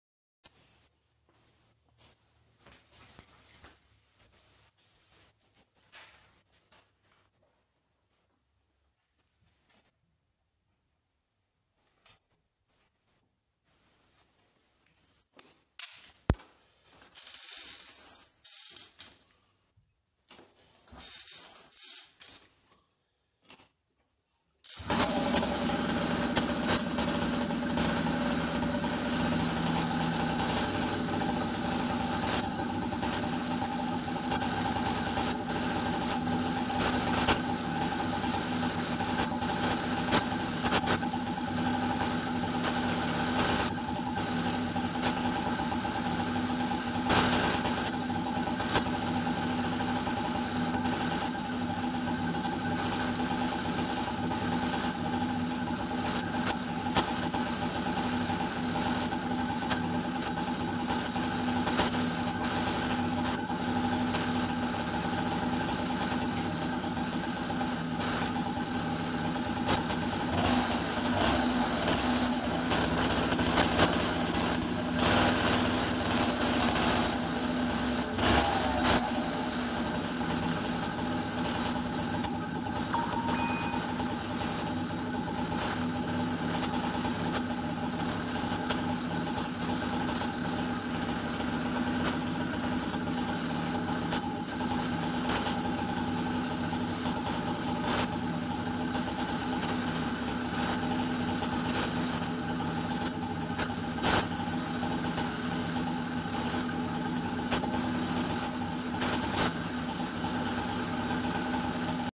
50mph motorized bike with simple 12v charging system
I put together my own charging system for motorized bicycle.